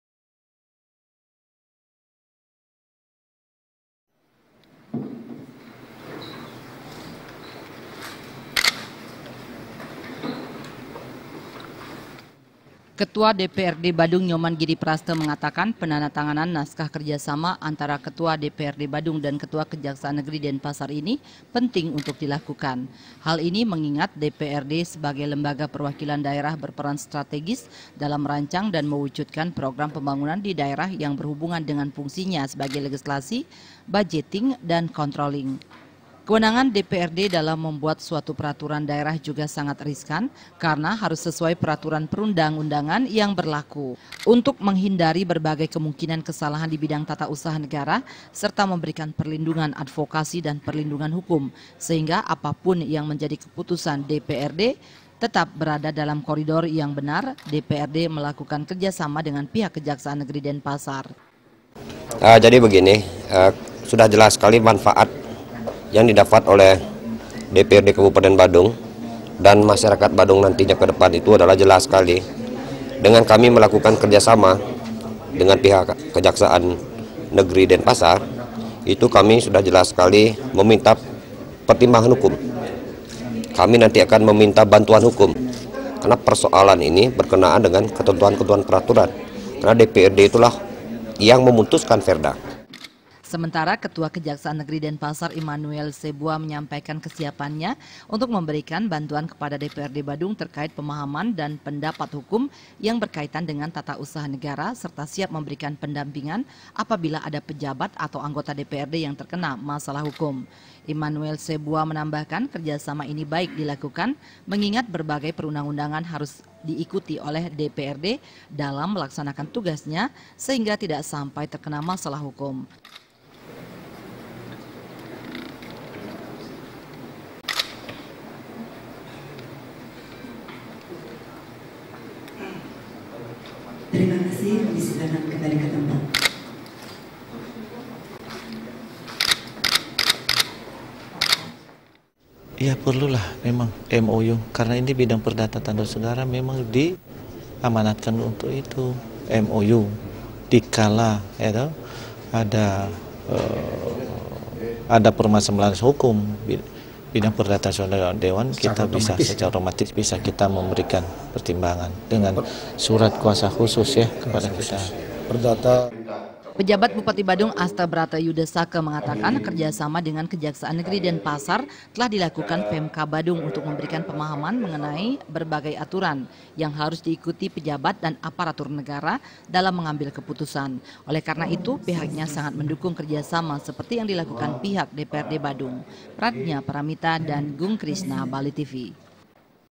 KERJA SAMA DPRD BADUNG DAN KEJARI DENPASAR
PERAN DPRD DALAM PEMBANGUNAN DI SUATU DAERAH SANGATLAH PENTING. SEBAGAI LEGISLASI, DPRD MEMILIKI FUNGSI SEBAGAI PENYUSUN ANGGARAN DAN PENGONTROL, AGAR SEMUA PROGRAM DAN KEPUTUSAN YANG DIBUAT TIDAK MENYALAHI PERATURAN. UNTUK ITU, DPRD BADUNG MENJALIN KERJA SAMA DENGAN KEJAKSAAN NEGERI DENPASAR DI BIDANG PERDATA DAN HUKUM TATA USAHA NEGARA.